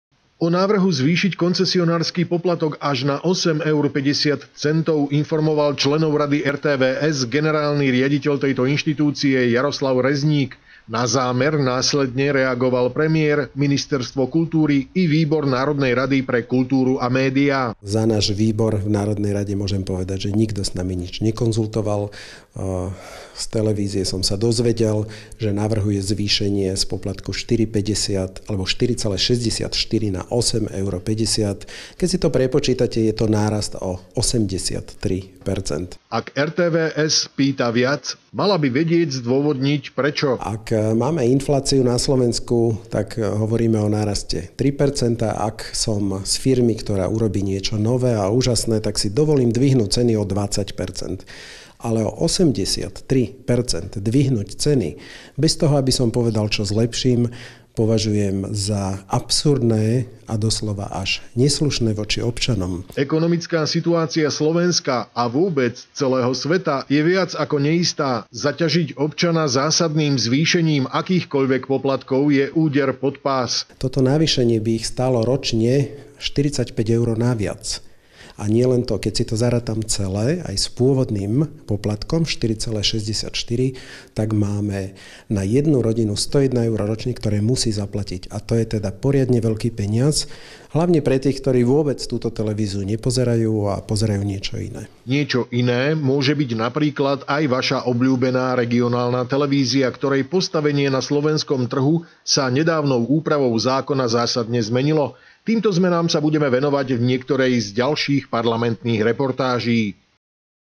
Vega Tv - Koncesionárske poplatky
O návrhu zvýšiť koncesionársky poplatok až na 8,5 eura informoval členov Rady RTVS generálny riaditeľ tejto inštitúcie -  Jaroslav Rezník. Na zámer následne reagoval premiér,  ministerstvo kultúry i Výbor NR SR pre kultúru a médiá.